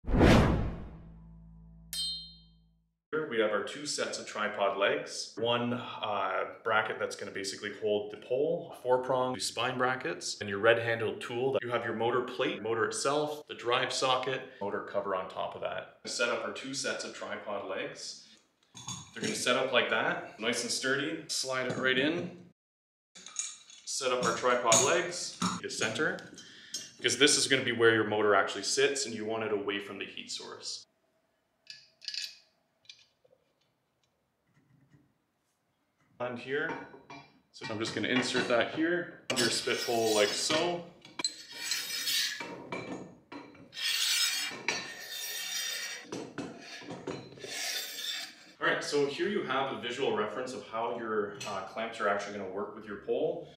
Pig Rotisserie Set-up in less than 60 seconds
Learn how to set up your PigOut Rotisserie in under 60 seconds